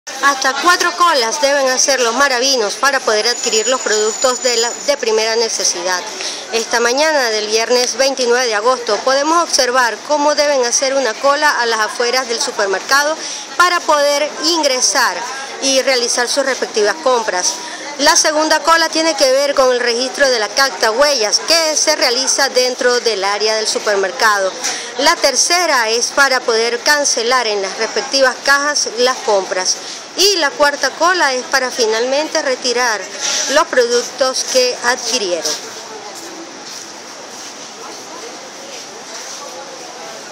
Hasta 4 colas deben hacer los marabinos para comprar en los supermercados
Cuatro colas deben hacer los usuarios de este supermercado para registrarse y comprar mediante el sistema de captahuellas.
Video: Patricia Quiñonez